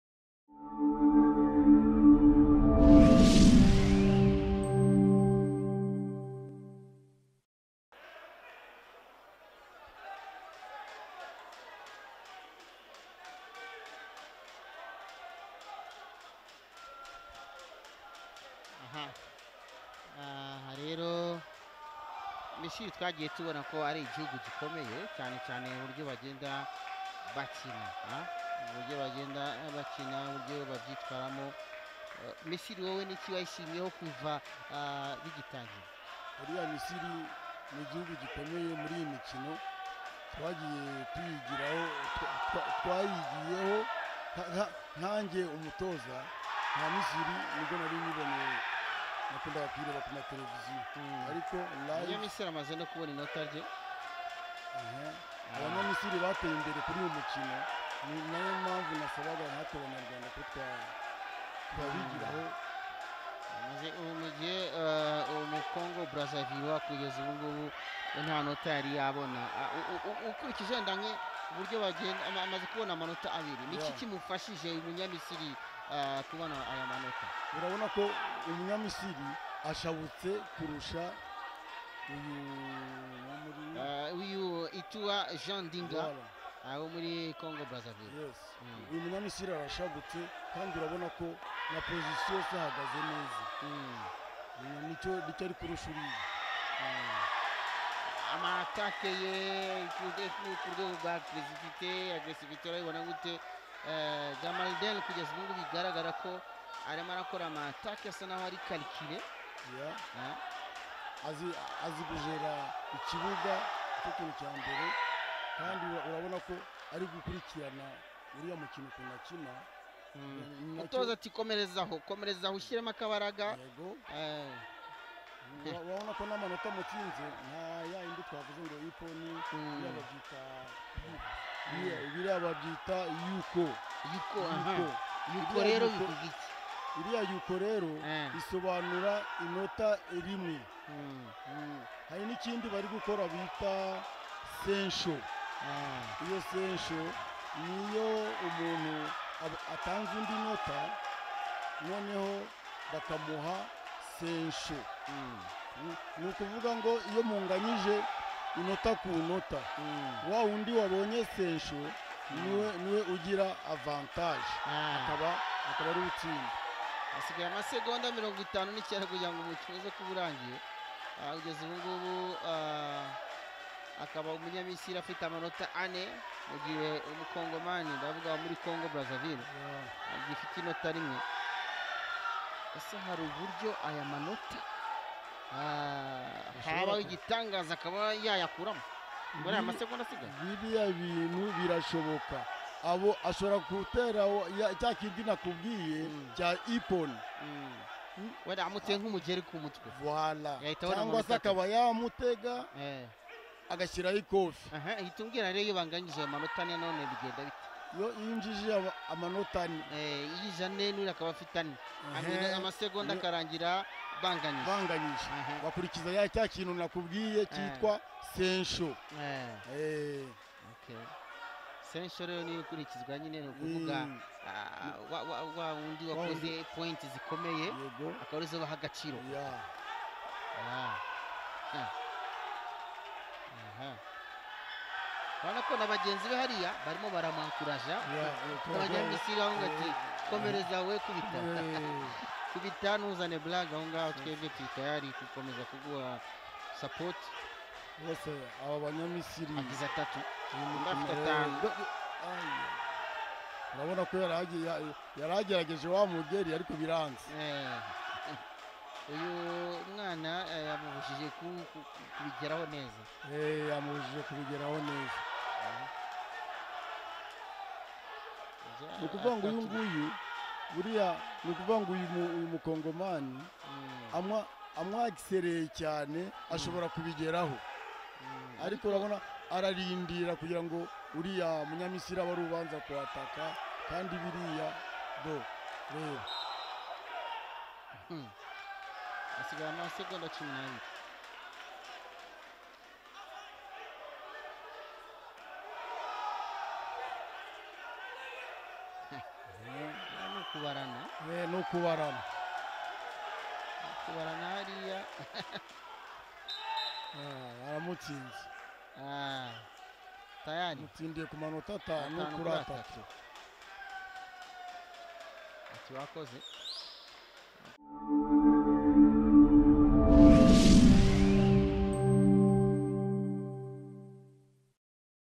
Itoua Jean Ndinga (CONGO) VS Gamaleldin Abdelrahman (EGYPT) || Junior Kumite Male -76 KG
We are uploading all videos of African Karate Championships 2018 held in Kigali - Rwanda from the 28th of August 2018 to the 02nd of September 2018.

"Sport4Change Media is leveraging the power of sport and media to change our planet for the better through our unique content."